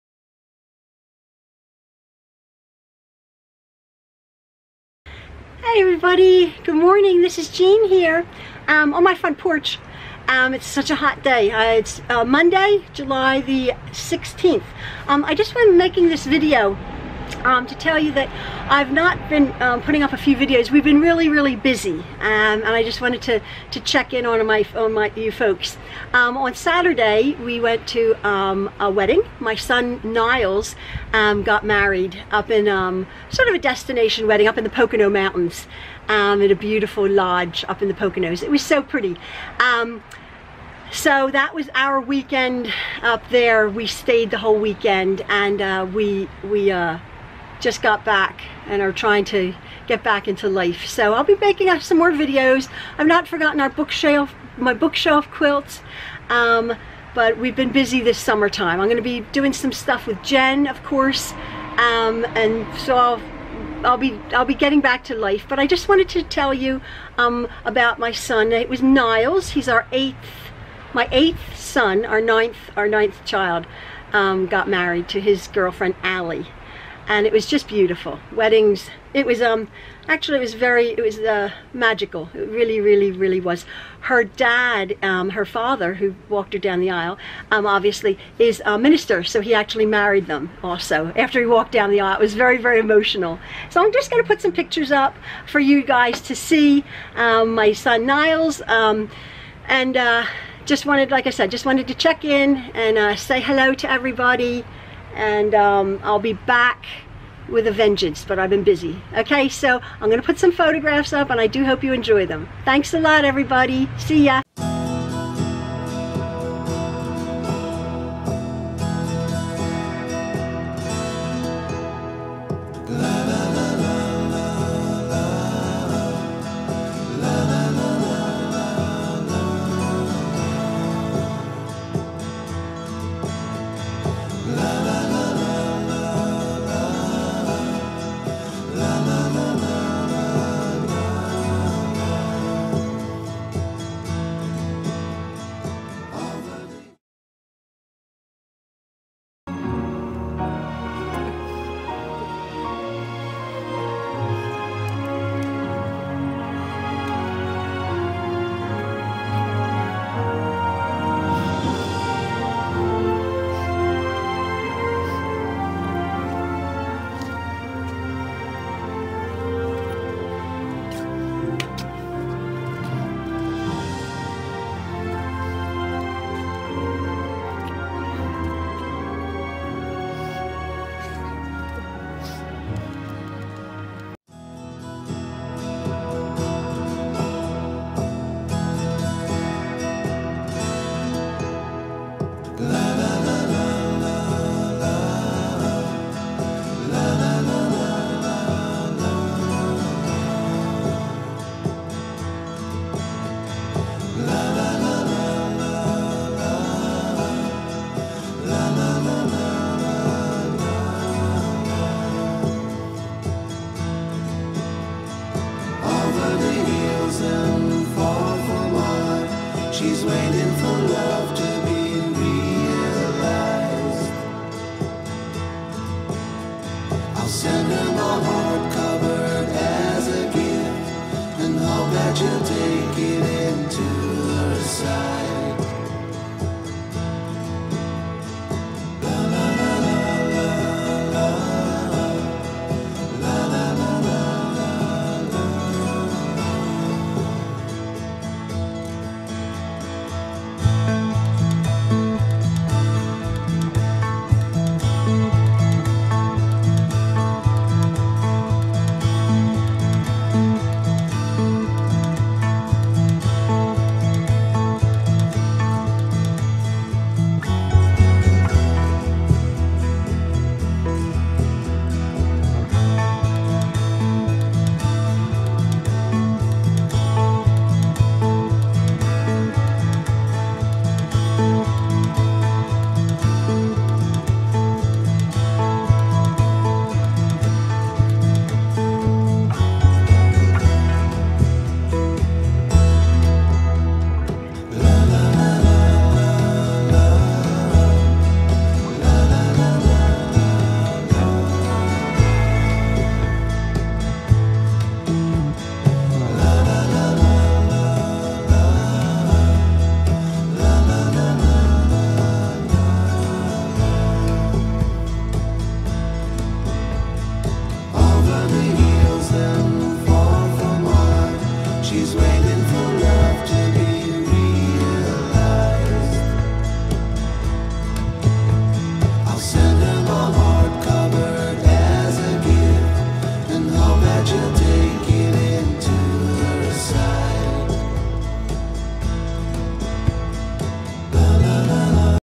Summer Break ~ Wedding Time
Been very busy lately ~ most of all with our son's wedding!  Hope you enjoy the picture...I'll be back soon, with Jen, to finish some projects and talk about some others!  (I'm trying a new movie editing program...don't judge!!  LOL!  Its a bit all over the place...not as easy as it looks!  But I'll try to get better)  LOL!